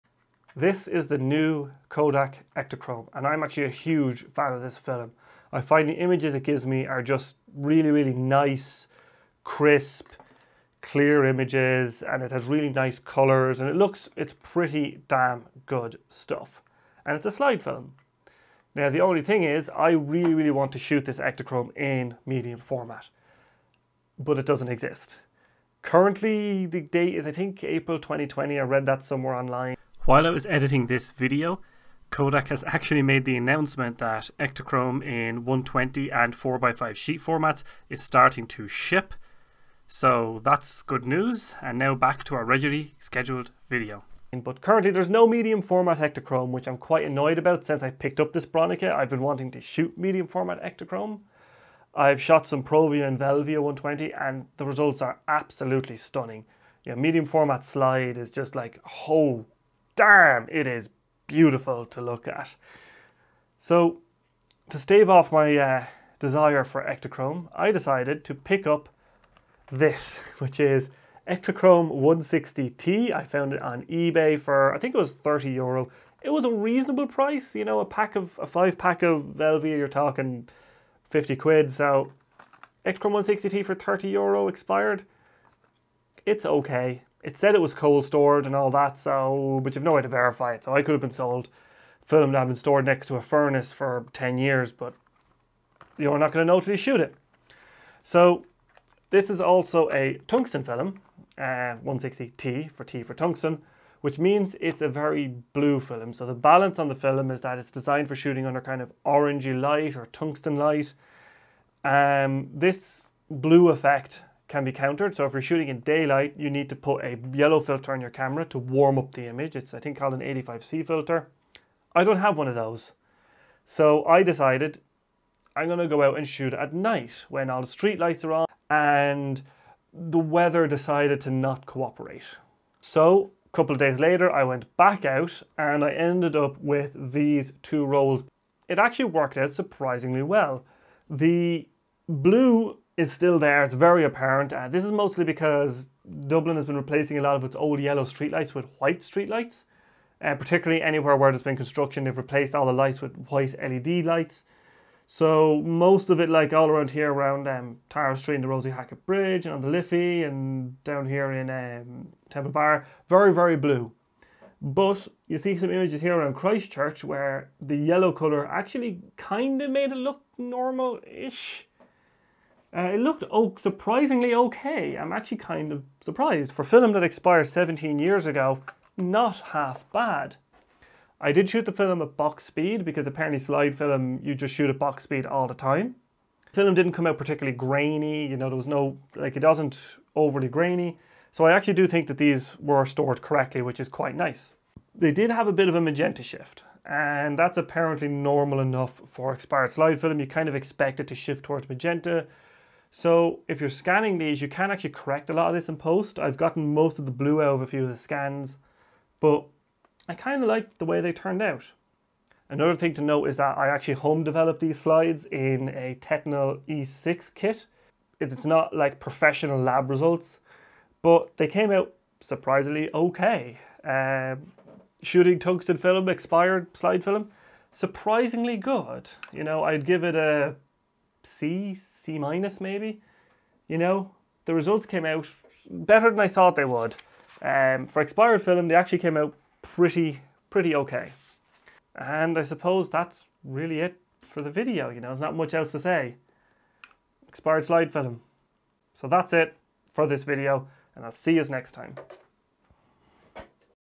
Using Expired Ektachrome 160T
I bought this film that expired in 2002 on ebay for some fun. It came out reasonably well for a film expired 17 years ago.